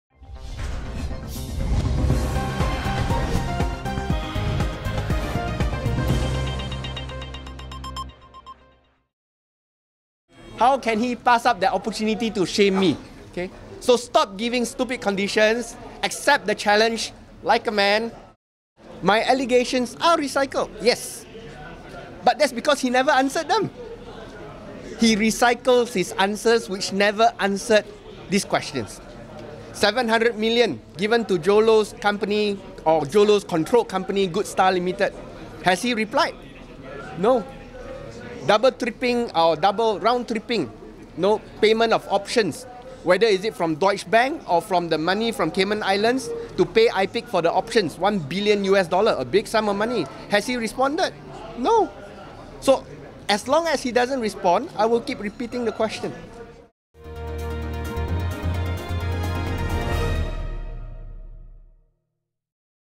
PUA TO ARUL KANDA: STOP GIVING STUPID EXCUSES, BE A MAN
DAP lawmaker Tony Pua has told 1MDB president Arul Kanda Kandasamy to be “man enough” to accept his challenge to participate in a live talk show on issues concerning the state-owned fund, and stop giving “stupid excuses”.